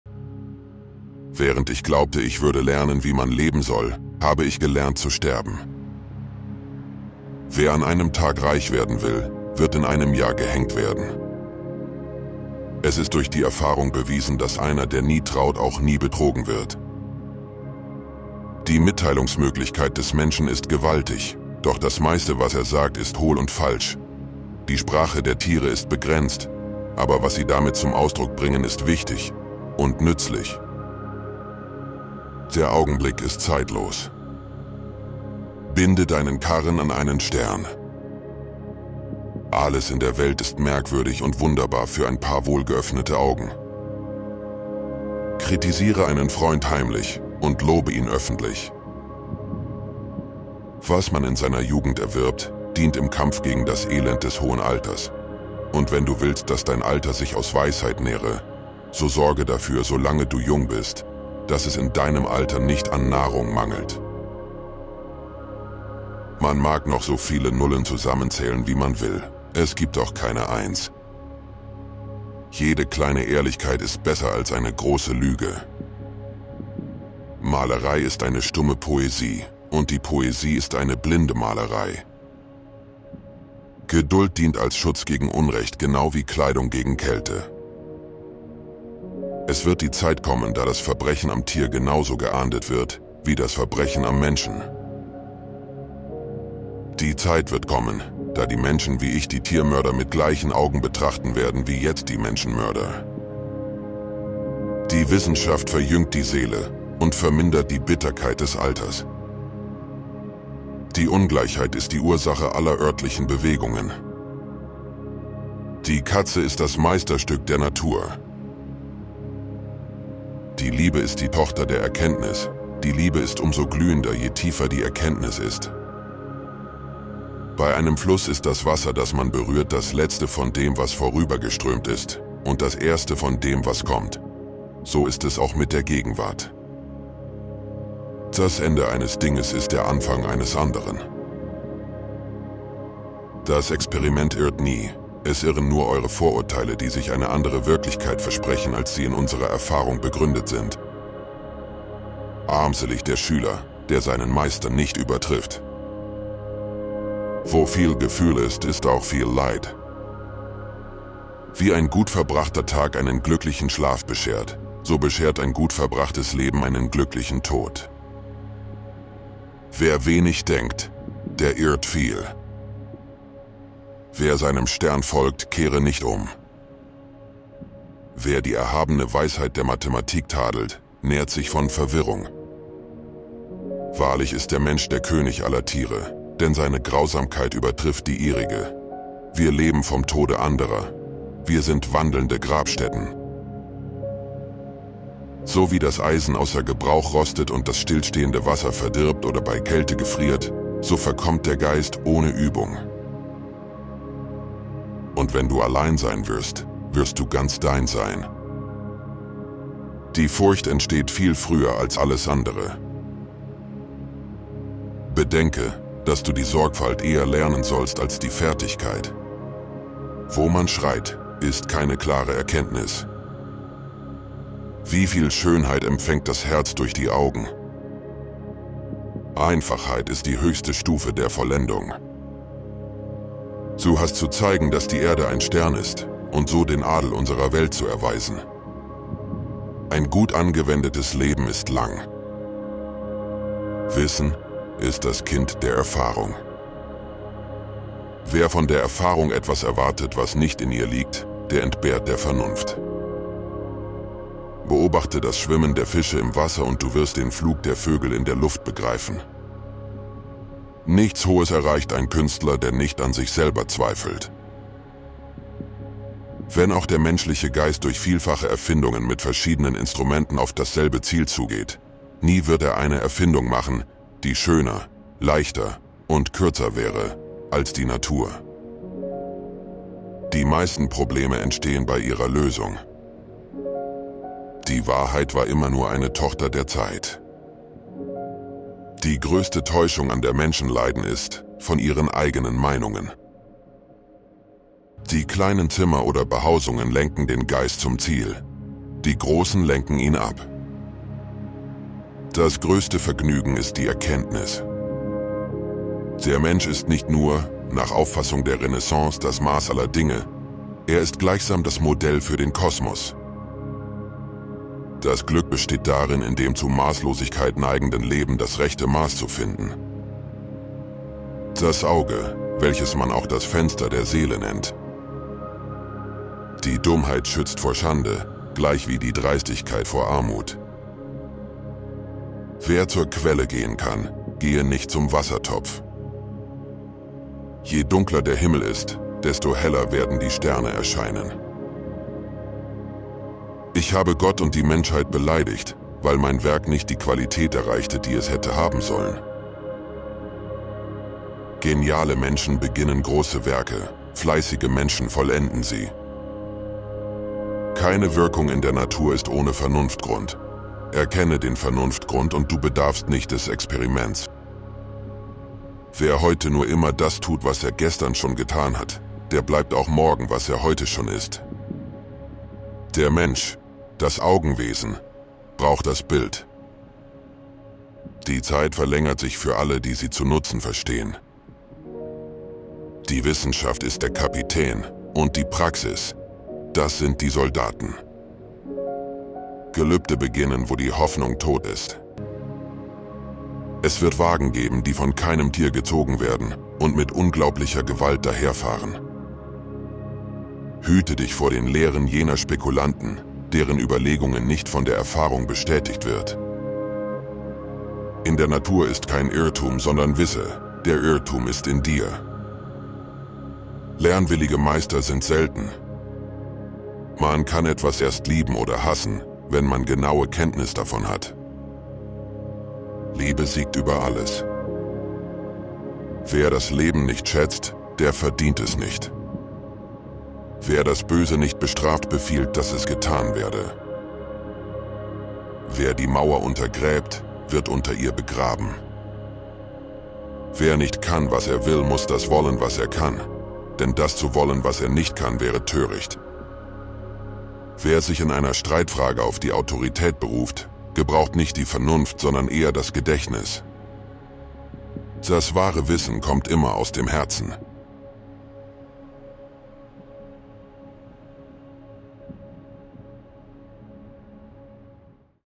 Leonardo da Vinci: Brillante Gedanken eines visionären Genies!
Leonardo da Vinci wurde am 15. April 1452 in Vinci, Italien, geboren. Er war ein Universalgelehrter der Renaissance, bekannt als Maler, Bildhauer, Architekt, Ingenieur und Erfinder. Da Vinci schuf einige der berühmtesten Kunstwerke der Welt, darunter **„Das letzte Abendmahl“** und **„Mona Lisa“**. Neben seiner Kunst war Leonardo auch ein brillanter Wissenschaftler und beschäftigte sich mit Anatomie, Geologie, Mathematik und Mechanik. Seine zahlreichen Skizzen und Entwürfe, darunter frühe Konzepte für Hubschrauber und Panzer, zeigen seinen visionären Geist. Leonardo da Vinci starb am 2. Mai 1519 in Amboise, Frankreich. Er gilt als Inbegriff des „Renaissance-Menschen“ und als einer der größten Genies der Geschichte.